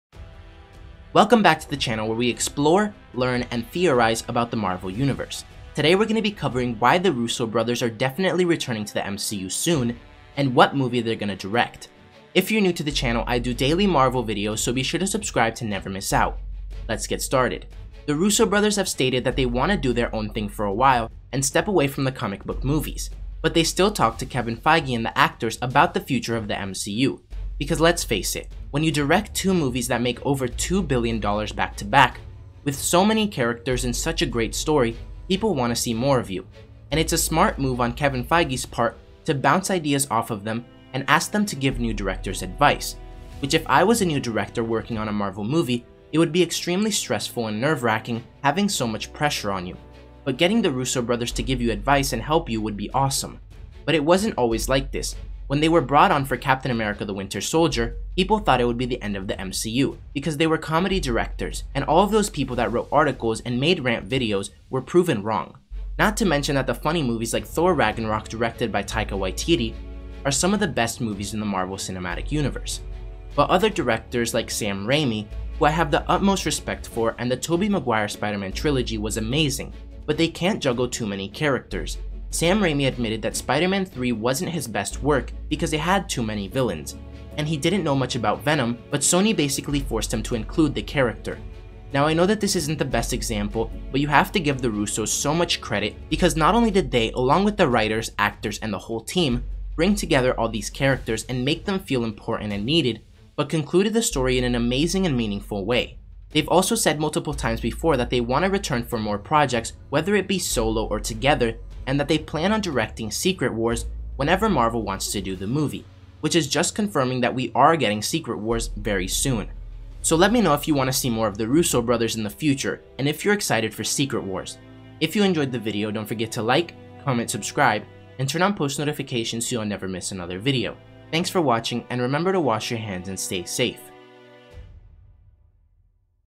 SECRET WARS IS COMING AND THE RUSSO BROTHERS WILL ONCE AGAIN DIRECT IT!!!
The directors of Avengers Infinity War and Endgame Joe and Anthony Russo will be back very soon to direct Secret Wars!